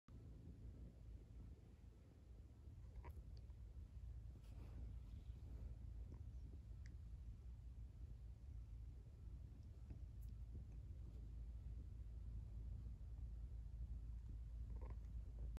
9/28/2021: All in bed together
Quietly trying not to wake honey with cats and cuddles.